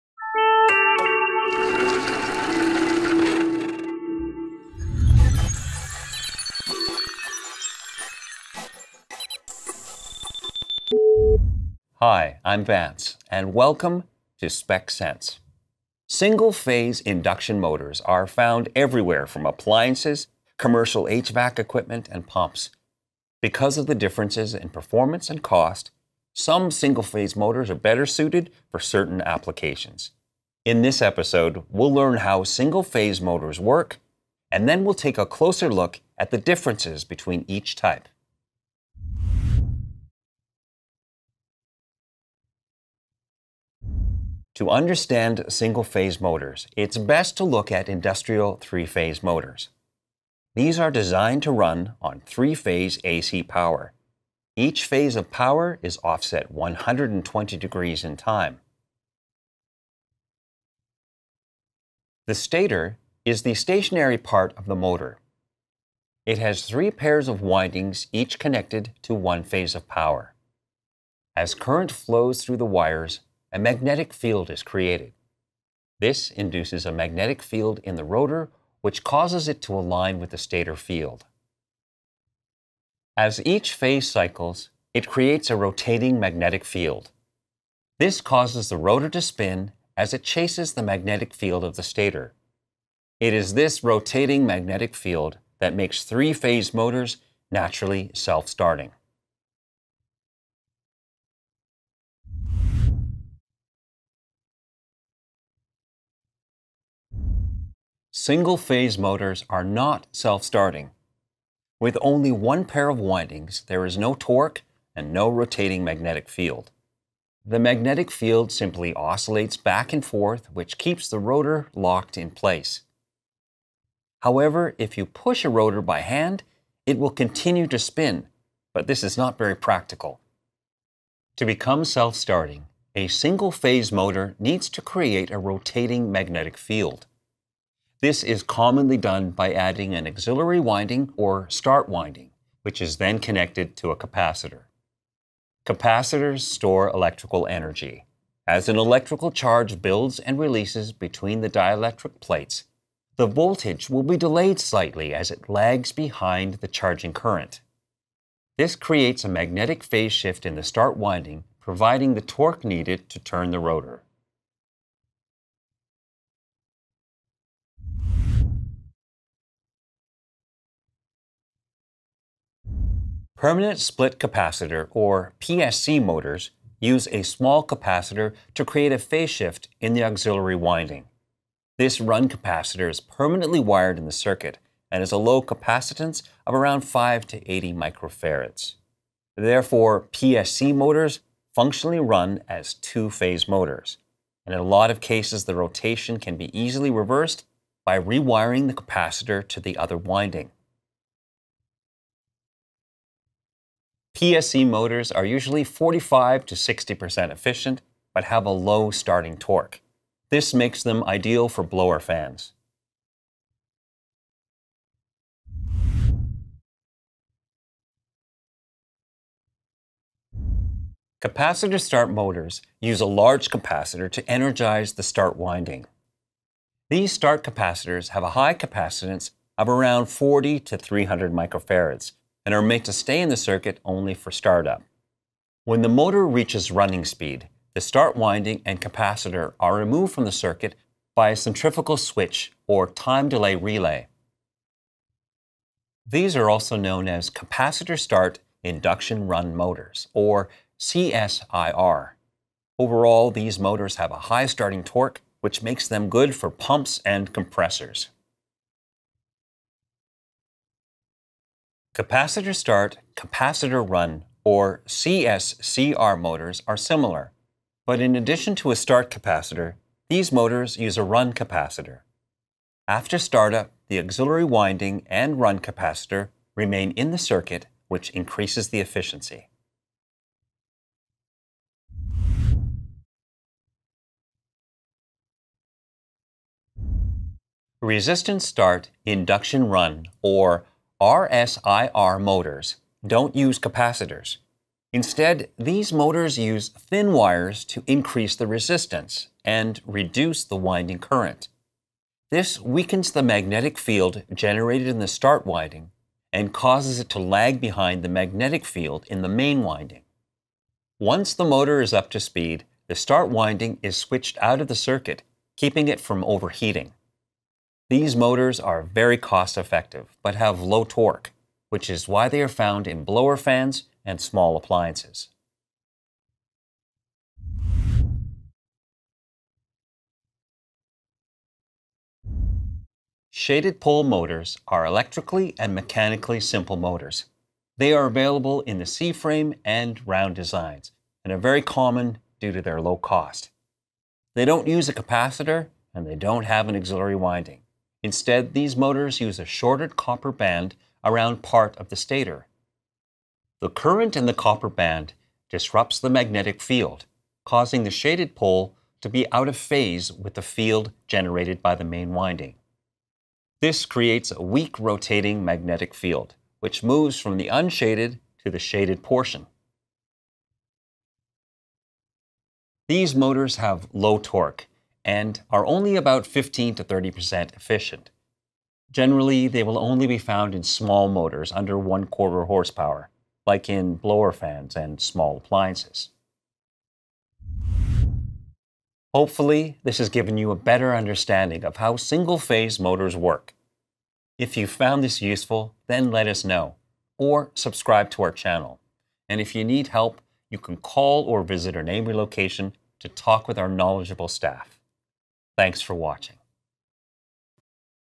How Do Electric Motors Work? | Spec. Sense
How does an electric single phase motor work? In this video, the basic principles of an AC induction motor are explained. First we'll cover the differences between 3 phase and single phase motors and show how the start winding is used to create a rotating magnetic field. Then we'll look at the differences between Permanent Split Capacitor, Capacitor Start, and Resistant Start motors, and will explain the unique function of Shaded Pole Motors.

This includes the motors used in pumps, furnaces, air conditioners, condensers and appliances.

00:00 Intro
00:39 Three Phase Motors
01:42 Single Phase Motors
02:48 Permanent Split Capacitor Motors
03:36 Capacitor Start Motors
04:43 Resistance Start Motors
05:28 Shaded Pole Motors